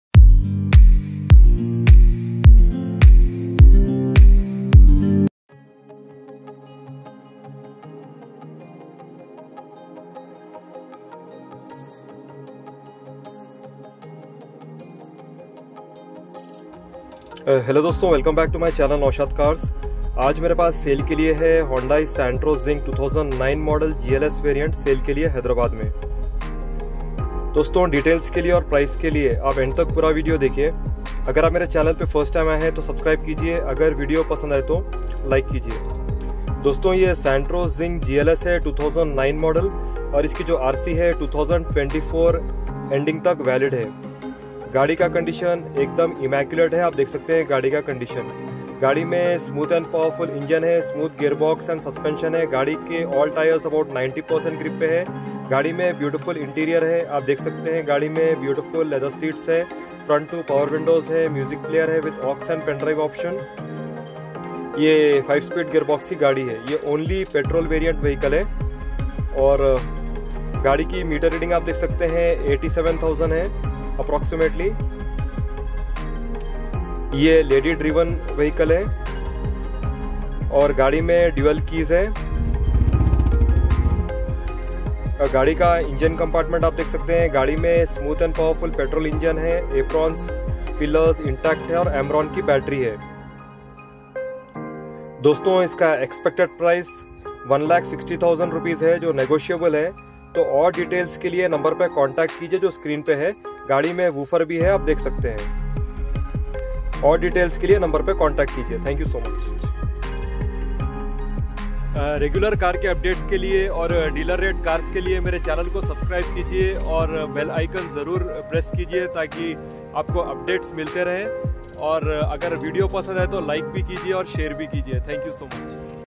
Santro Xing GLS 2009 Immaculate Condition Sale in Hyderabad
Santro Xing 2009 model 2009 registration, RC valid till 2024 ending, petrol, Immaculate condition, 2009 registration, fully loaded with front two power windows, power steering, chilled a.c, original car, smooth and powerful engine, good tyres and battery, RC valid, music player, all details in video.